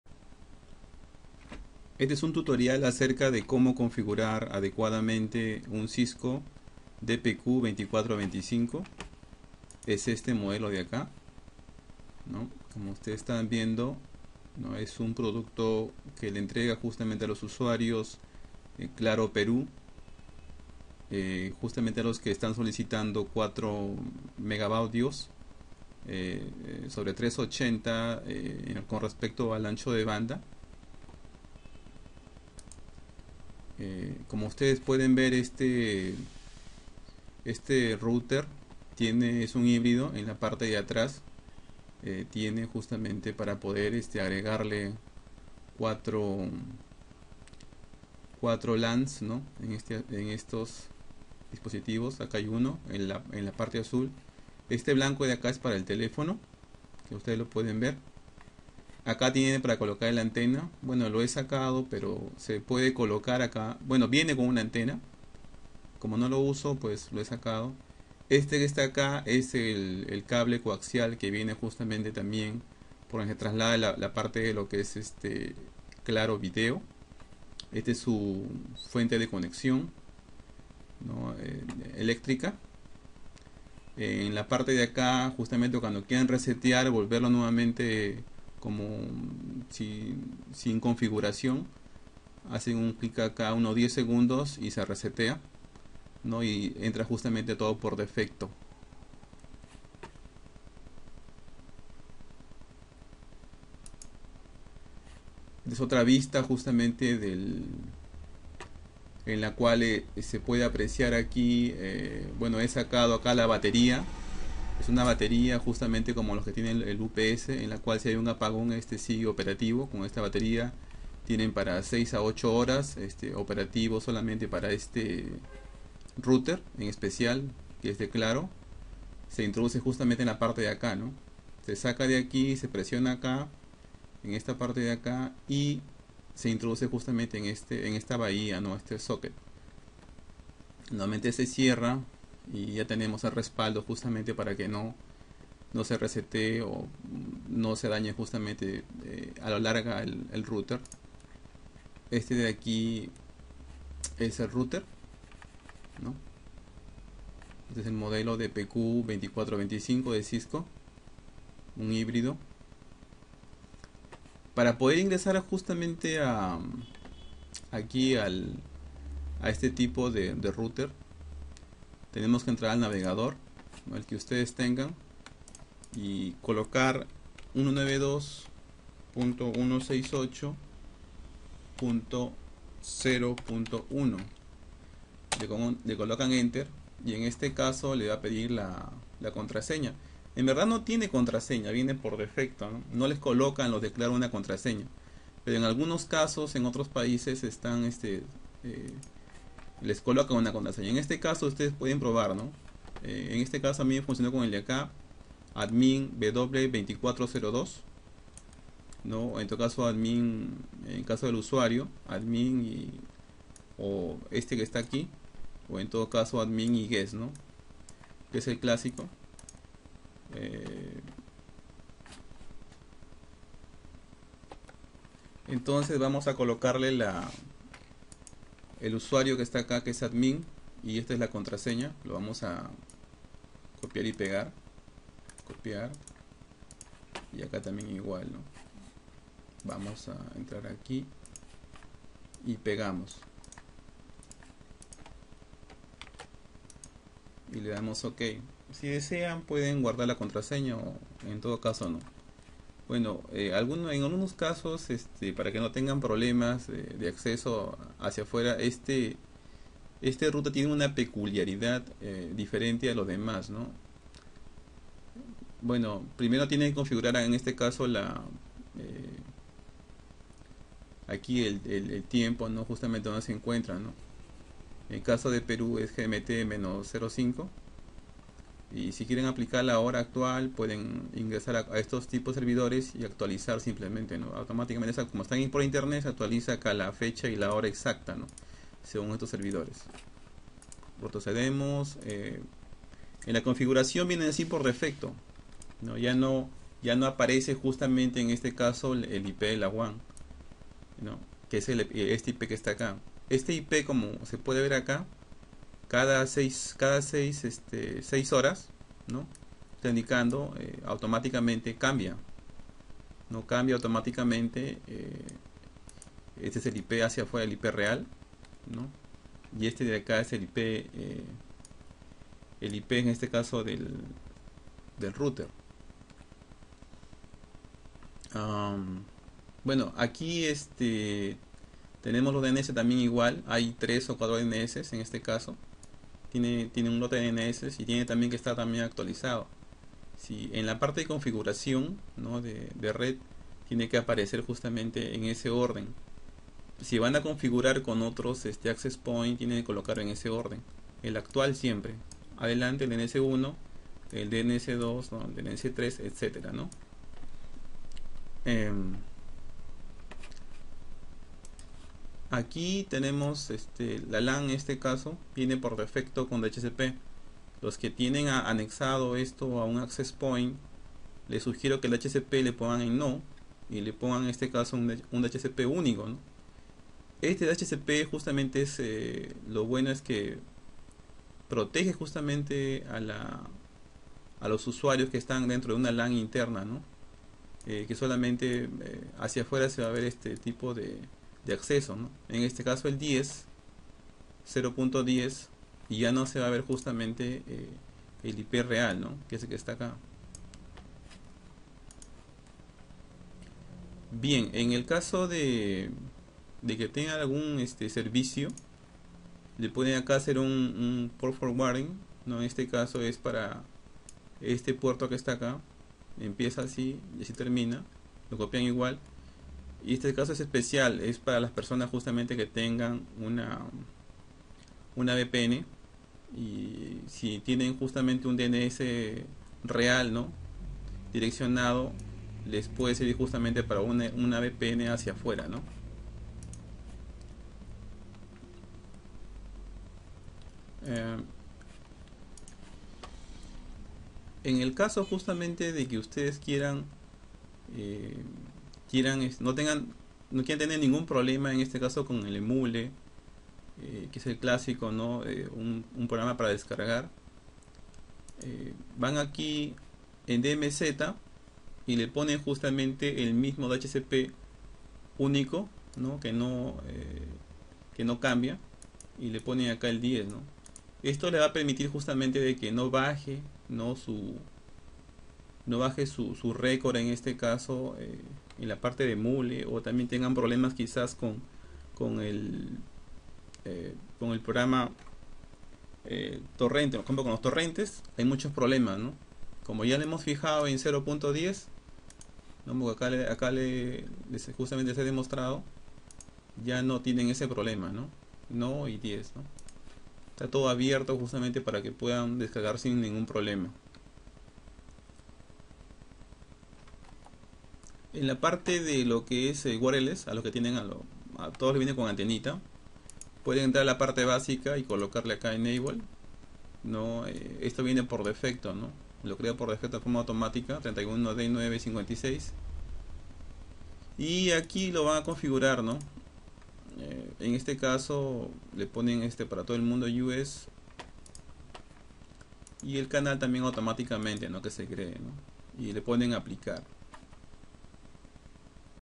Como Configurar un Router Cisco DPQ2425 Claro 2017
Bienvenidos!
Ante la propuesta de Youtube de establecer metas mensuales. Voy a disminuir el soporte o respuestas a las personas que busquen soporte.
Router cedido por claro - Peru
Descripcion
Fotos
Ingresar con usuario y contraseña
Como abrir puertos para VPN
Como hacer que funcione con emule y torrents, para que no bajen su record y tengan más accesos a los servidores.